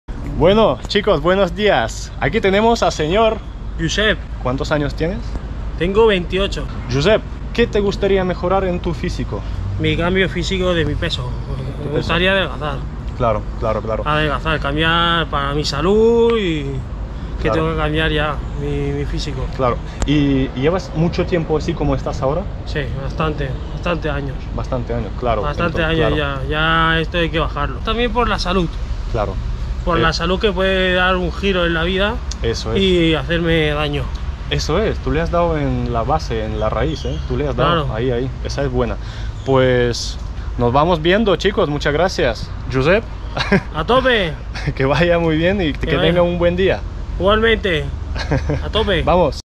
"Gente diferente" con Dima Muzhikov #3 Josep
José un chico de 28 años que lleva mucho tiempo en este estado físico y le gustaría mejorar! En lo que le vamos a ayudar!